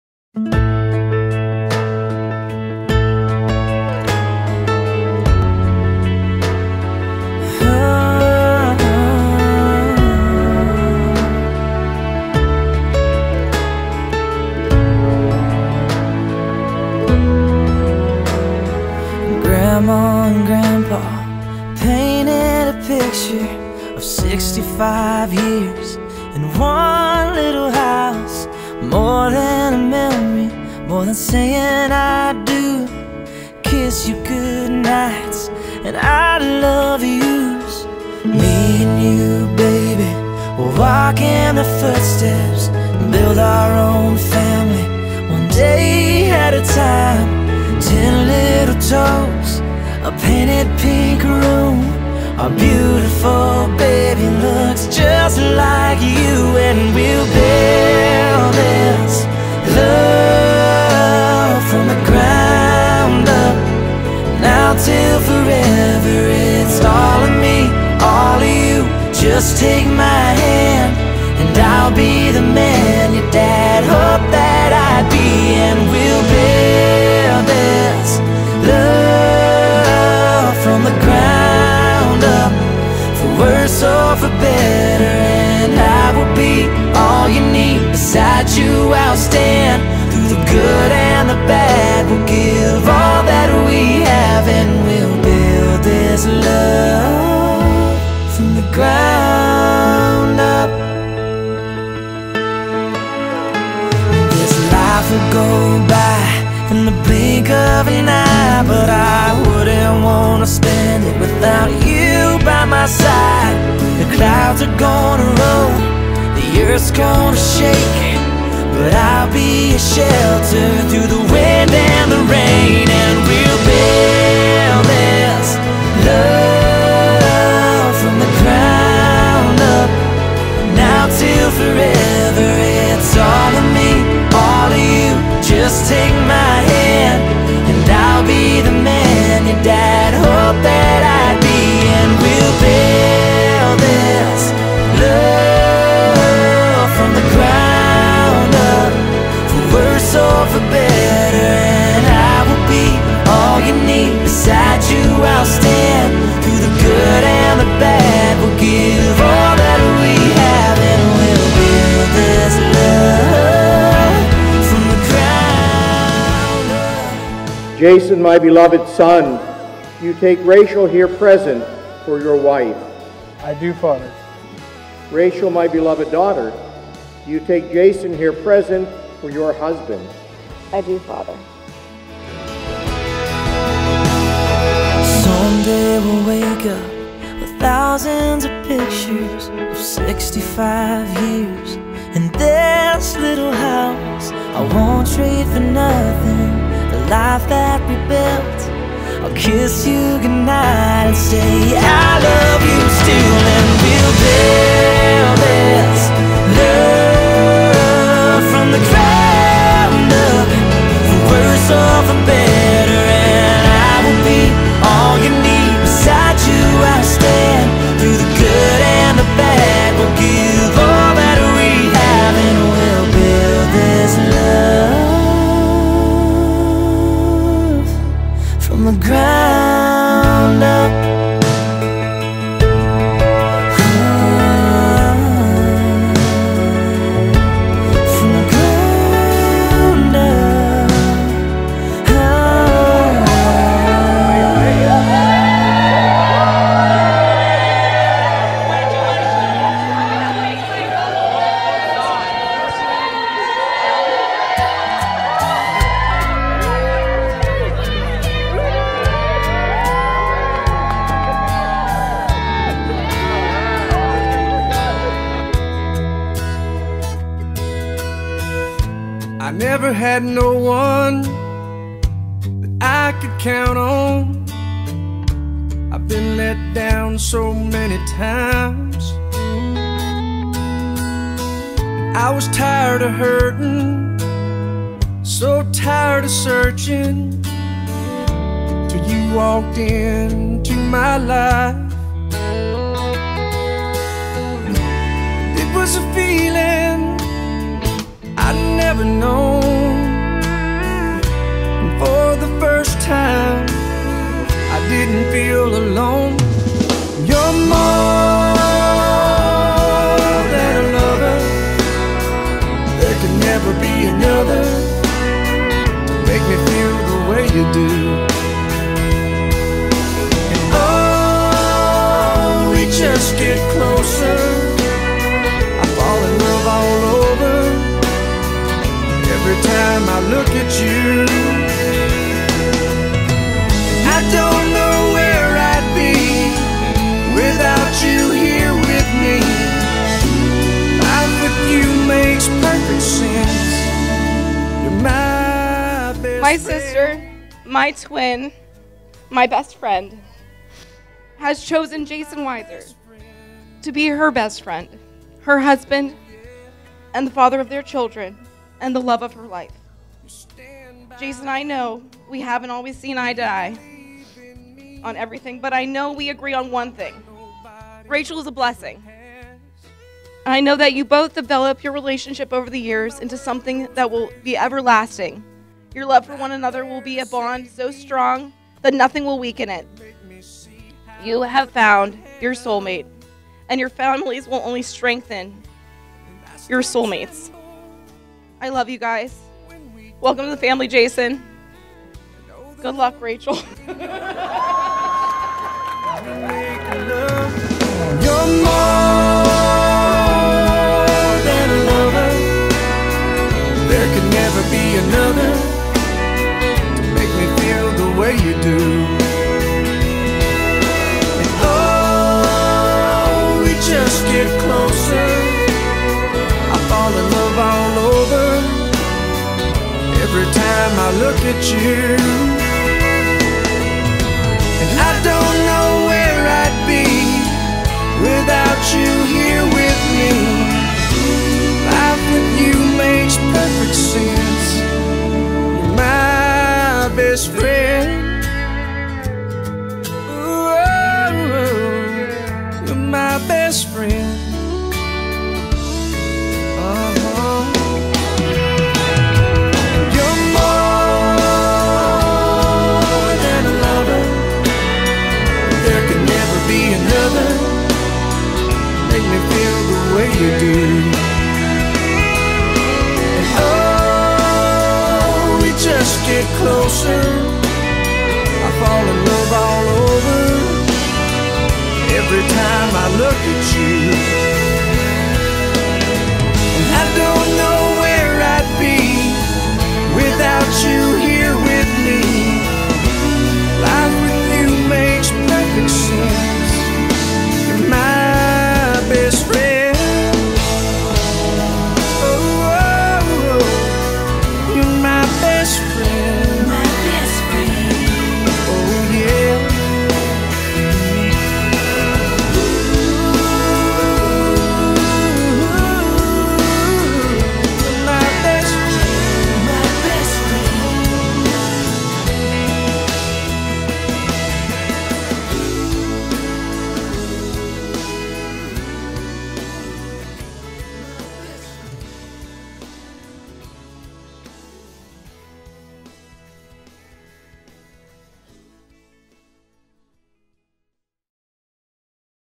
Jason and Rachel Wedding Highlight Film
Ceremony: Our Lady of Lebanon, 2759 N. Lipkey Road, North Jackson, Ohio 44451

Reception: The Embassy, 5030 Youngstown-Poland Road
Boardman, OHIO 44512

Photos: B&O Station,  530 Mahoning Ave, Youngstown, OH 44502 & Mahoning Ave. Bridge

Aaron Pangio Wedding and Event Films
Youngstown Ohio Wedding Video